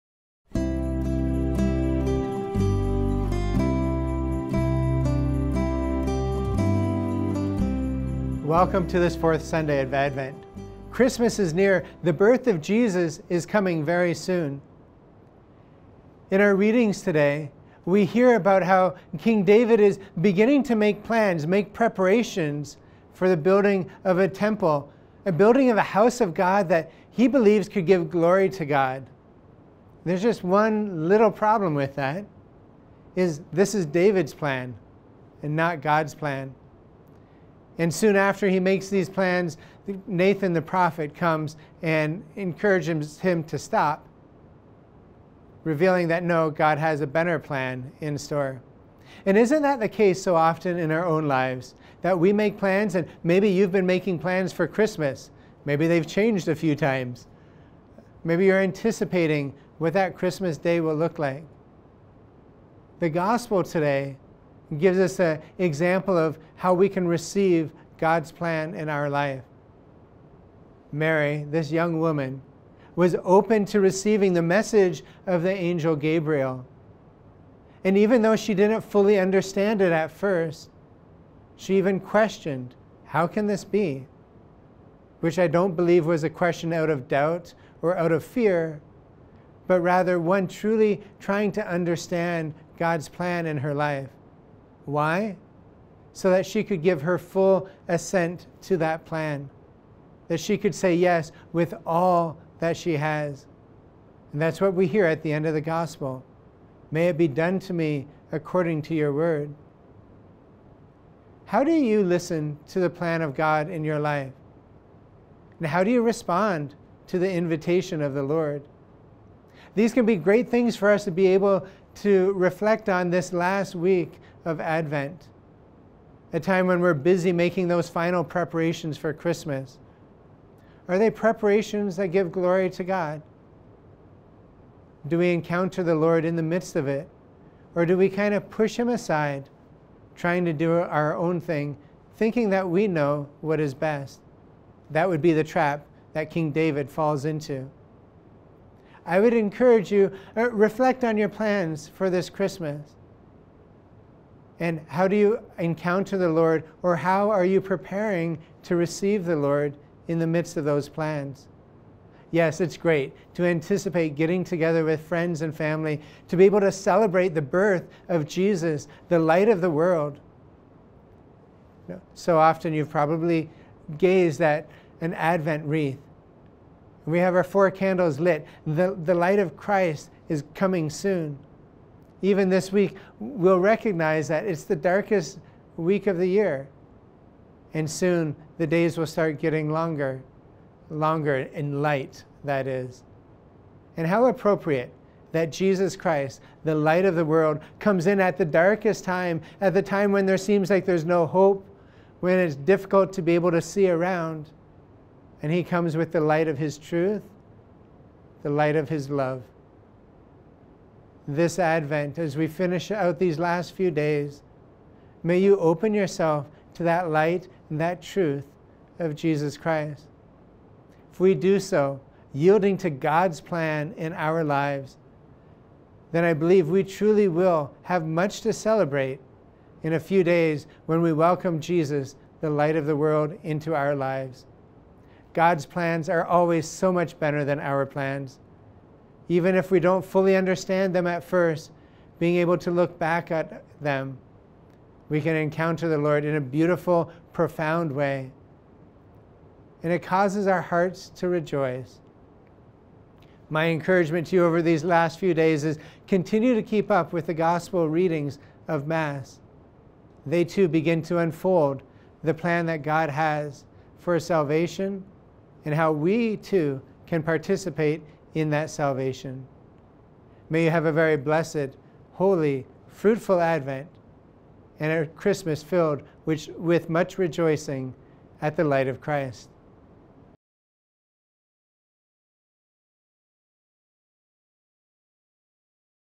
4th Sunday of Advent: Fr. Jonathan McElhone, TOR: “Jesus Christ, the Light of the World”
Father Jonathan McElhone, TOR, director of the Priestly Discernment Program at Franciscan University of Steubenville, says Mary’s example in the Annunciation shows us how to say yes to God’s wonderful plan for us and open ourselves to his light and truth instead of following our own plans.